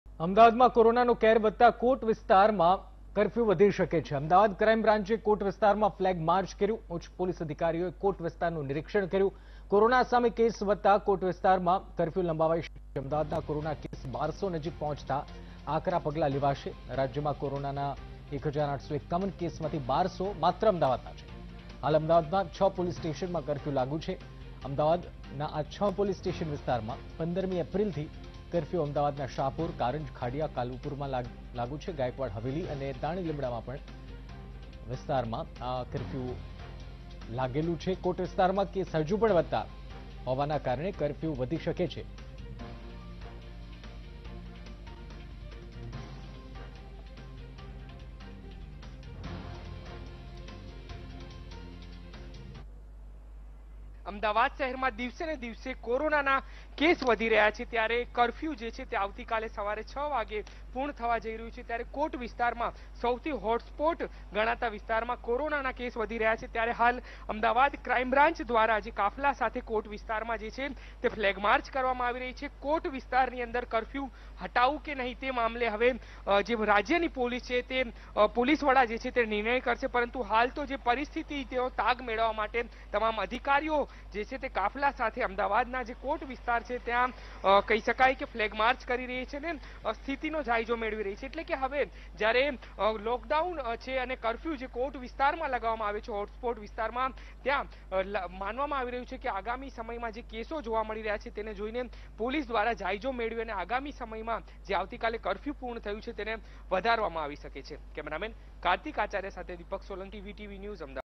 કોરોનાના કહેરના કારણે Ahmedabad માં કર્ફયૂ વધારવા આજે લેવાઈ શકે છે નિર્ણય | VTV Gujarati
કોરોનાના કહેરના કારણે  Ahmedabad માં કર્ફયૂ વધારવા આજે  લેવાઈ શકે છે નિર્ણય